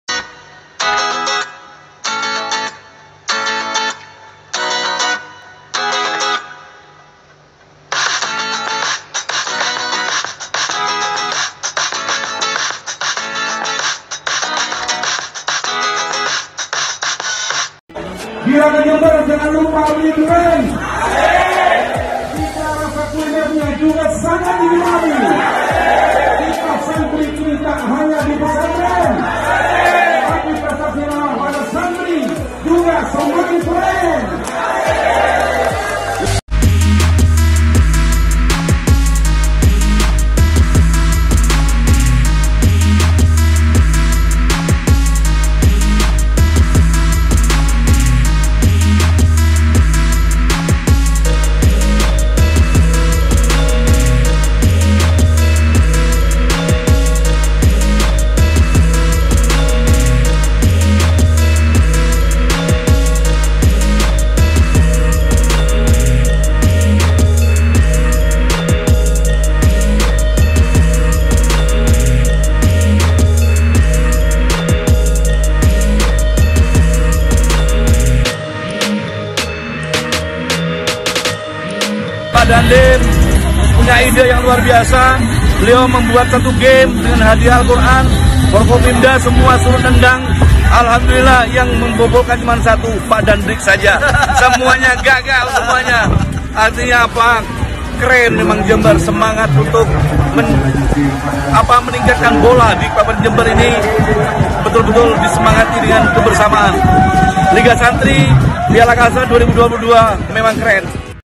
Bupati Hendy: Liga Santri Jember Upaya Lahirkan Bibit-bibit Baru Pesepak Bola Andal
Keseruan mewarnai acara pembukaan Piala Kasad Liga Santri PSSI Senin sore (4/7/2022) di Stadion Notohadinegoro di Kreongan, Patrang, Jember.

Tepatnya, pada saat istirahat babak pertama, Bupati Jember Ir H Hendy Siswanto, ST, IPU dan jajaran Forum Koordinasi Pimpinan Daerah (Forkopimda) mendapat tantangan dari Komandan Distrik Militer (Dandim).

Satu per satu ditantang menendang bola ke gawang. Kipernya diambil dari penonton. Penonton yang berani maju mendapat hadiah Al Quran.

"Ternyata, semua yang menenadang bola gagal. Hanya Pak Dandim yang bisa membobol gawang," kisah Bupati Hendy sambil tertawa lepas. "Senang sekali dengan kebersamaan ini. Semua bersemangat untuk memajukan sepak bola di Kabupaten Jember. Caranya, dengan kolaborasi yang sangat baik seperti ini."

Adapun tujuan penyelenggaraan Liga Santri ini agar para santri cinta bermain bola. Lahir sehat, batin pun kuat. Lebih dari itu, mereka kelak bisa berprestasi lebih lagi di luar pesantren.

"Semoga dengan adanya Liga Santri ini bisa membawa semangat baru, dan menghasilkan bibit-bibit pemain sepak bola andal Kabupaten Jember. Proses regenerasi di persepakbolaan Jember dapat berjalan," pungkas Bupati Hendy, optimis. (*) Melia Hapsarani YSQ/MTS          Video: @hendysiswanto.hs/Habib YSQ